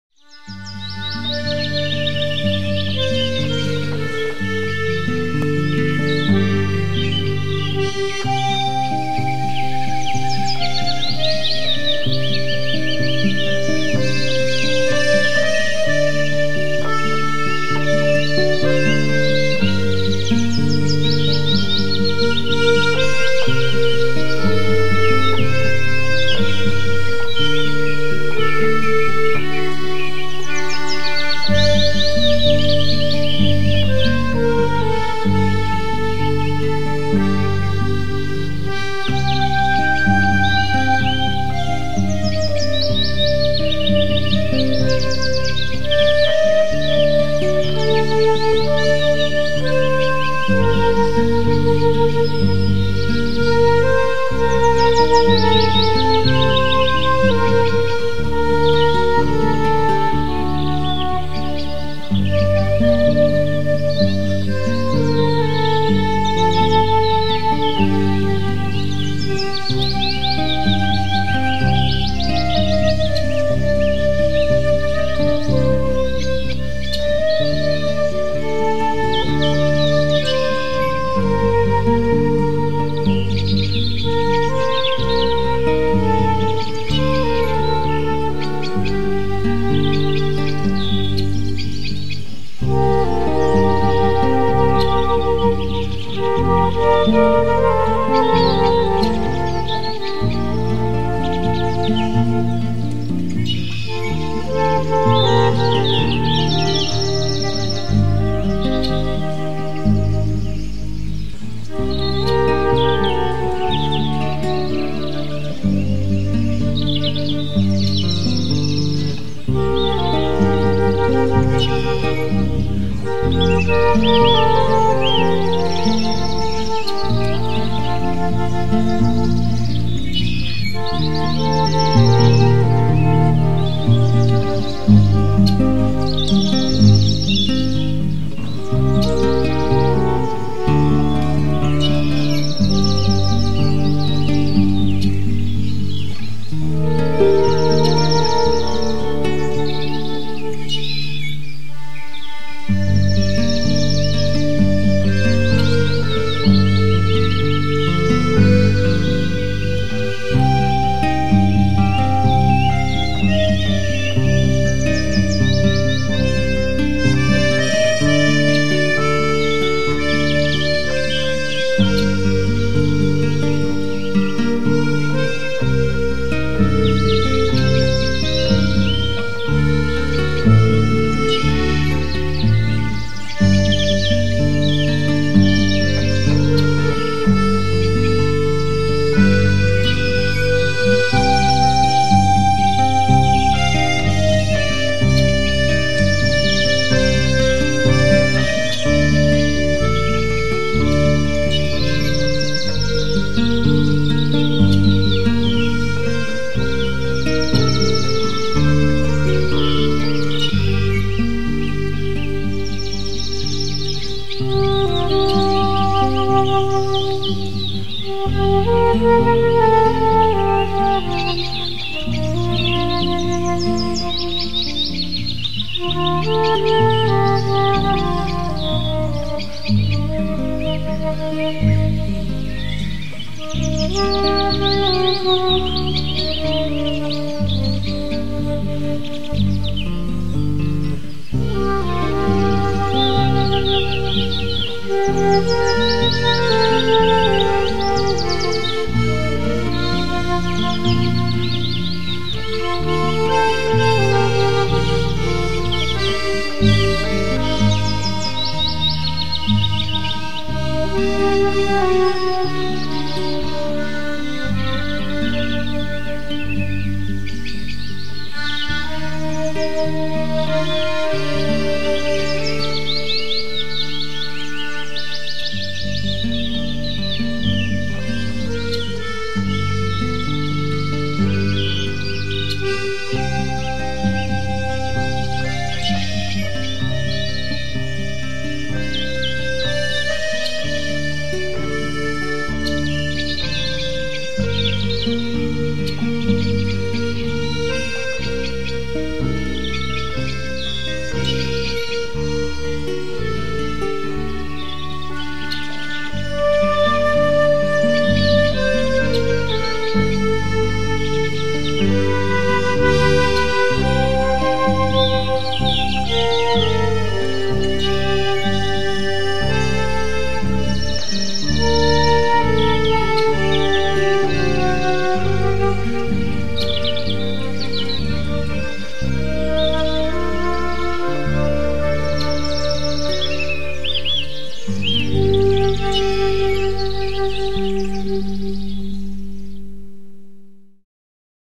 Beaux oiseaux, Saint-Fabien-de-Panet 12 oct. 2012
Voilà quelques oiseaux aux mangeoires en cette journée enrobée de quelques flocons de neige. Sur la musique interprétée par Cello, regardez les beaux oiseaux qui doivent manger pour se revitaliser par le temps plus froid. Plusieurs gros becs errants rivalisent avec les geais bleus gourmands. Merci pour vos commentaires.